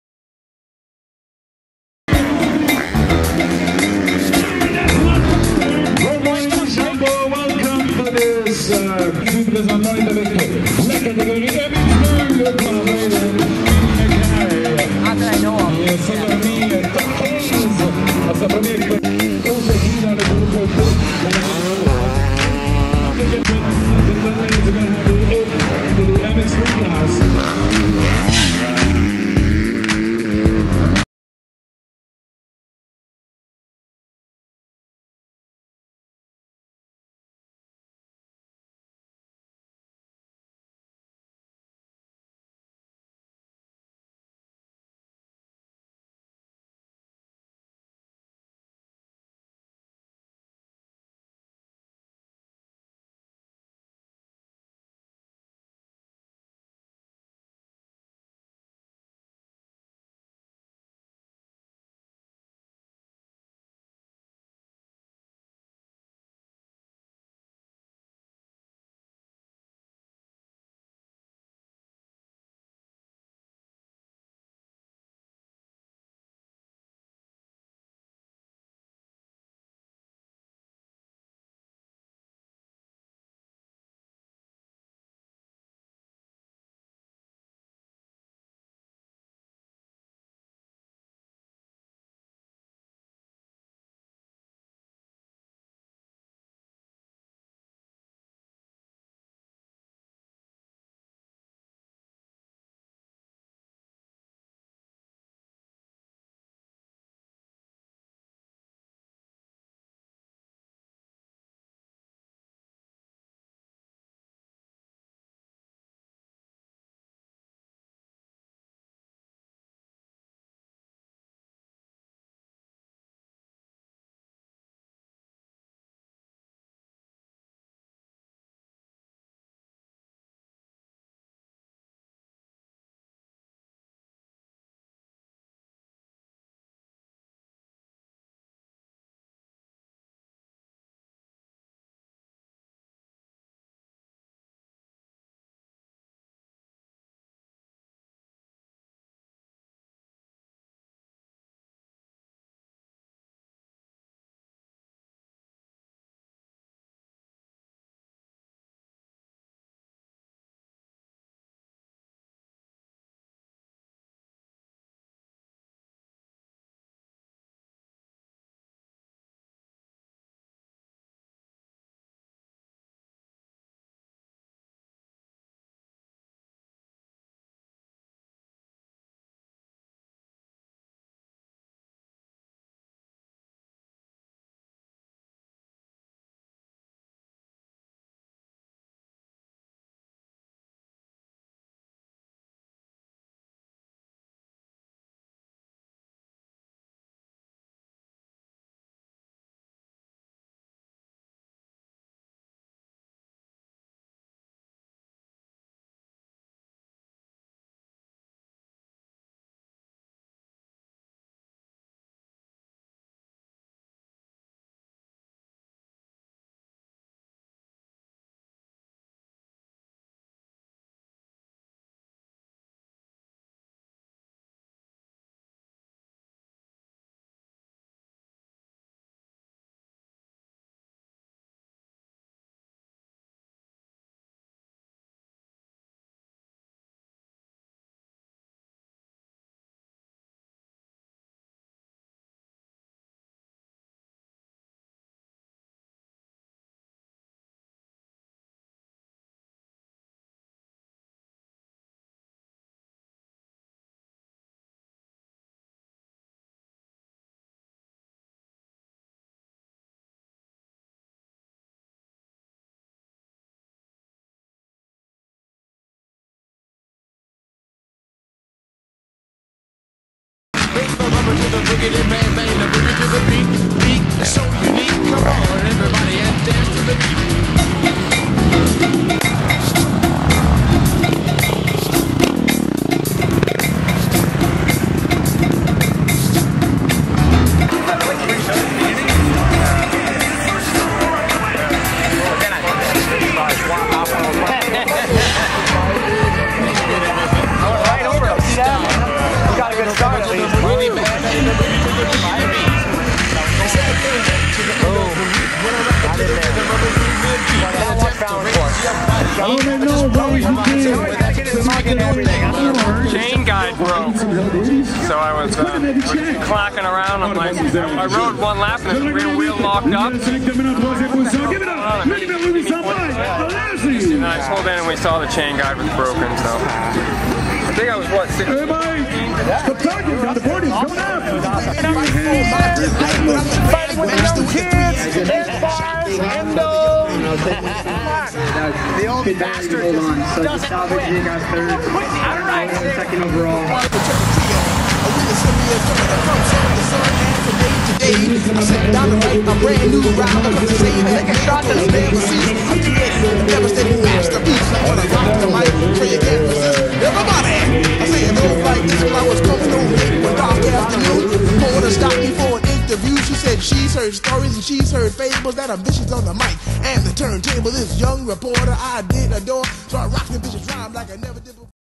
John Dowd and Keith Johnson Race the 2019 Deschambault Canadian MX National
#91 John Dowd and #401 Keith Johnson make the trip north to Deschambault, Quebec, Canada to race Round 7 of the 2019 Rockstar Energy Triple Crown MX Nationals at Motocross Deschambault.